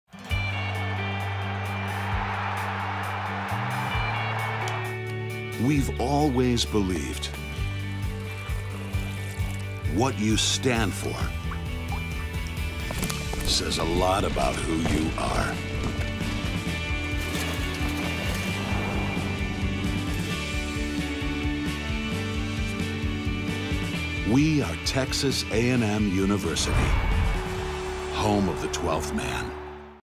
What You Stand For | Texas A&M 2013 Commercial
Aggies stand ready on the sidelines of Kyle Field every gameday, but the power of the 12th Man extends far beyond a playing field — it is echoed in Texas A&M's land-grant heritage, in the student body, in the unity, the loyalty and the willingness of Aggies to serve when called to do so.